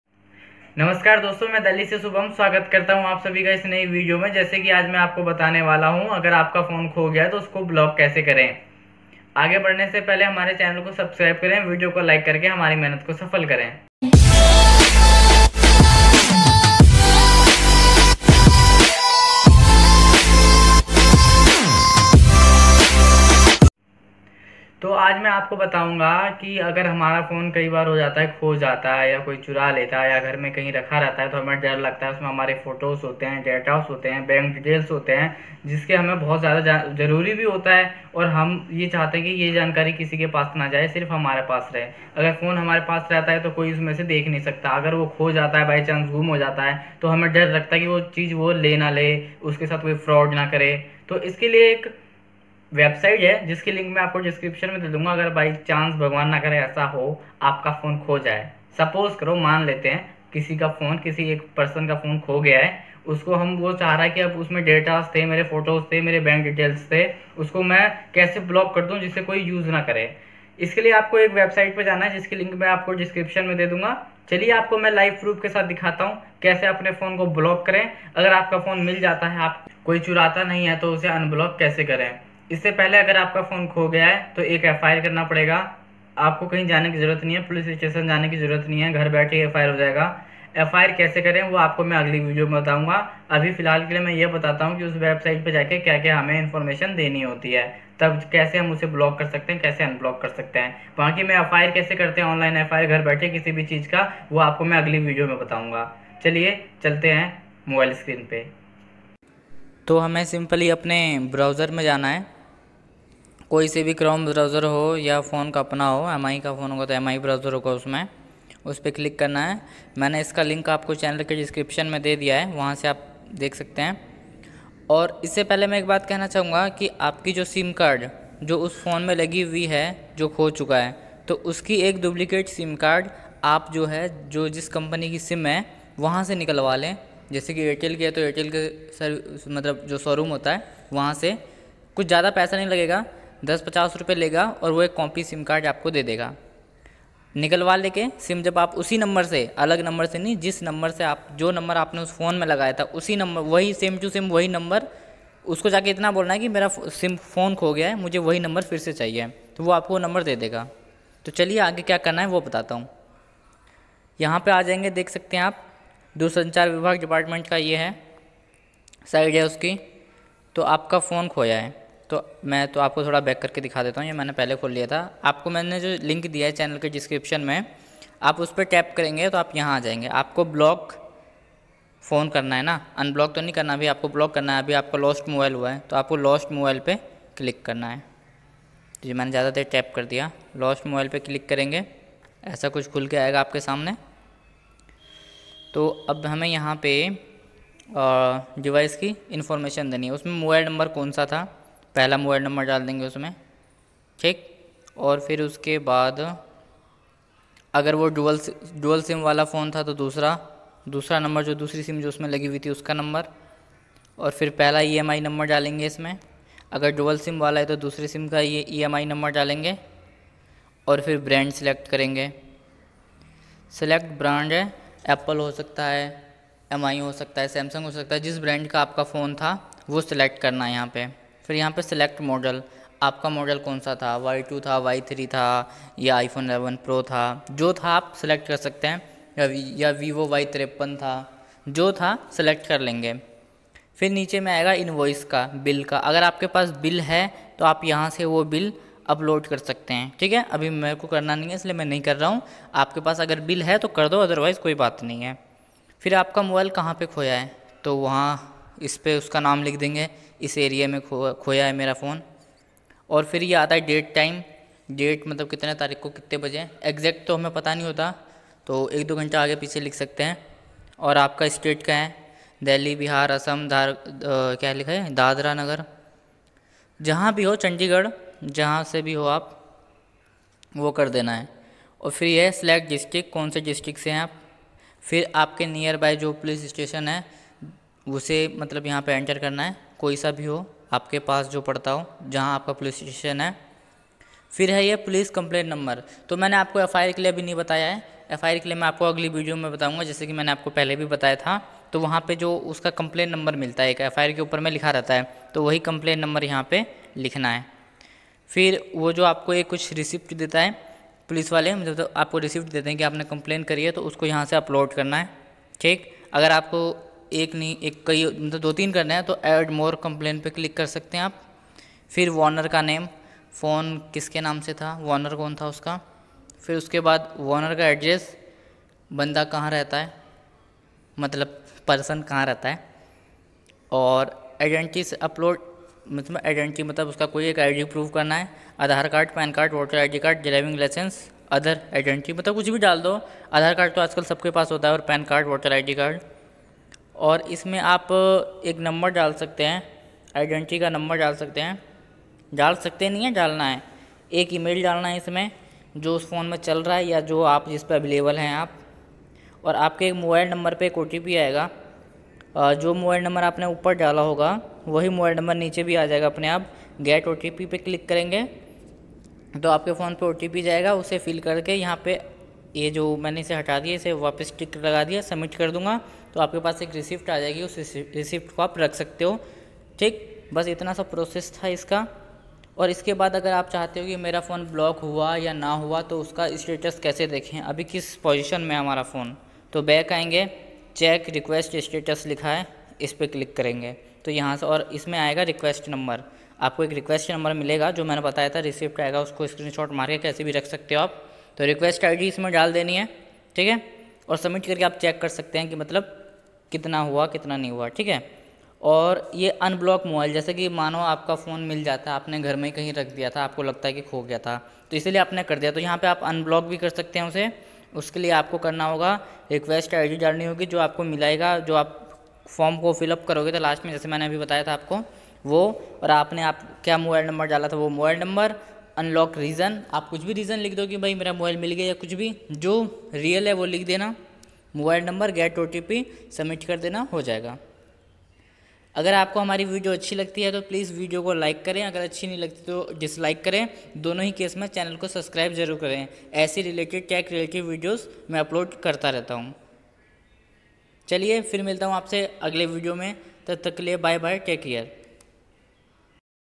How to Block Lost Mobile Phone- mobile block kaise kare \CEIR Stolen Mobile Tracking Portal
shubhamhemant How to Block Lost Mobile Phone- mobile block kaise kare \CEIR Stolen Mobile Tracking Portal

Tech.Sky🔛
Tech.Sky OFFICIAL" is a Youtube Channel, and this is the Official Telegram Channel Of
Our Discussion Group:-

agar aapka mobile chori ho gya ya kho gya to aap usko online block kr skte h jisse ki koi aapka phone use na kr ske. agar bad me aapka phone mil jata h to aap usko unblock b kr skte h

ye indian government ki portal hai yha lost or stolen mobile ko block kr skte h